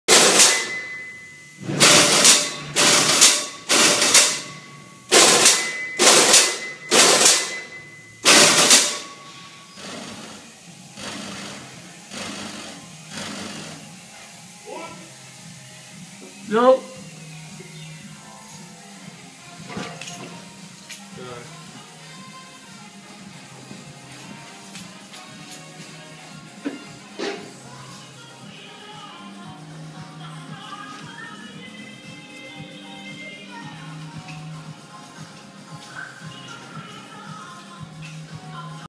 Yamato 14 head Multihead weigher for sale incl Platform
1991 Yamato Multihead weigher for sale still in good working condition including platform